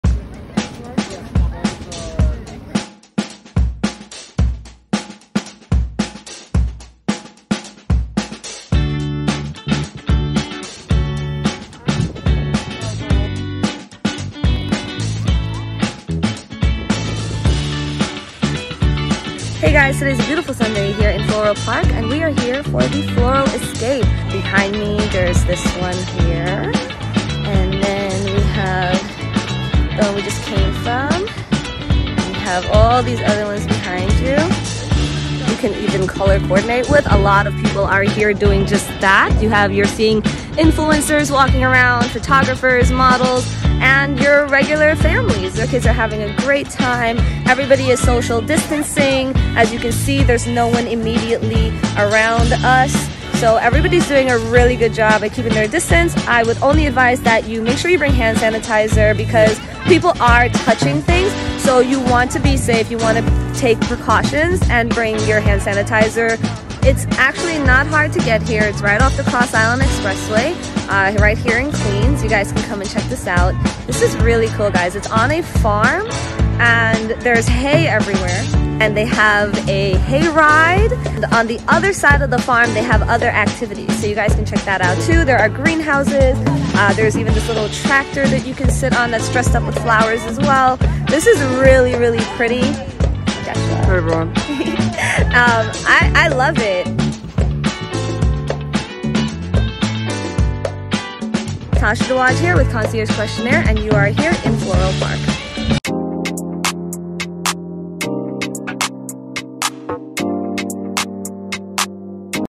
URHere at The Floral Escape's Winter Escape
URHere with ConciergeQ's Tasha Dwhaj at The Floral Escape's Winter Escape in Queens, New York. ​Take a stroll through the Gingerbread Village, stop by and smell the Candy Cane Rose Garden or snap a selfie with Santa in his throne chair at The Winter Escape.